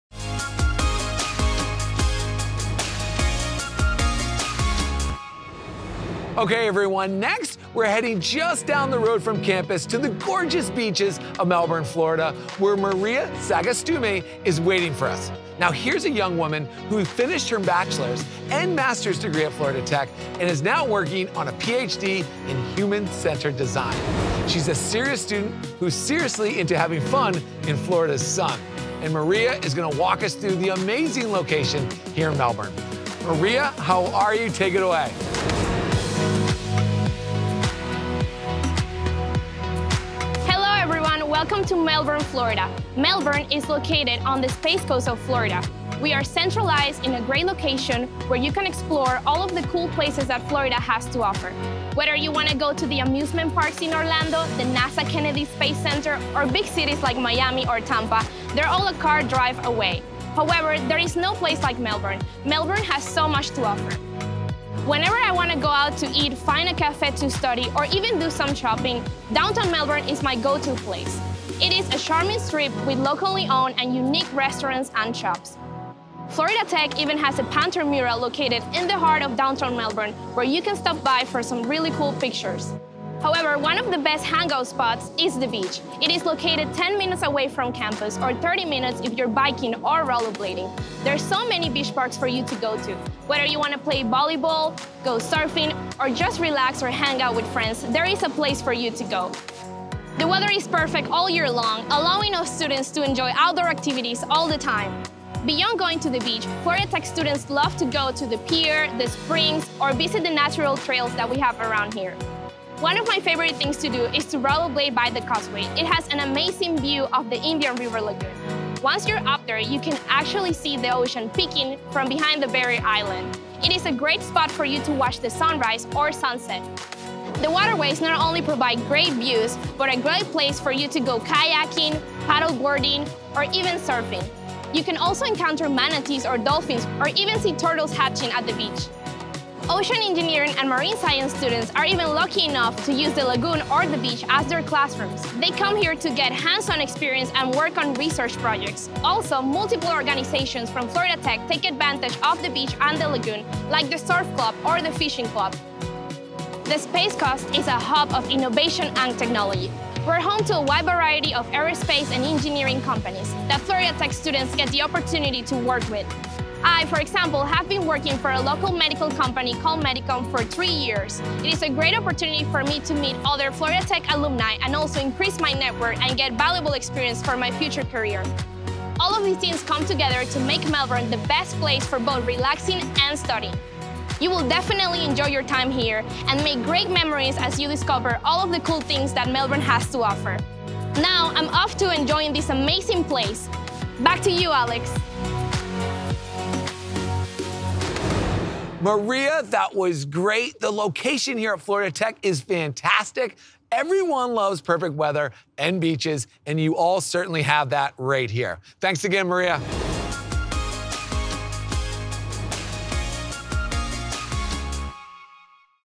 You Can’t Beat Florida Tech’s Location | The College Tour
Florida Tech's Maria Sagastume walks us through the school's fantastic Melbourne beach location in this video!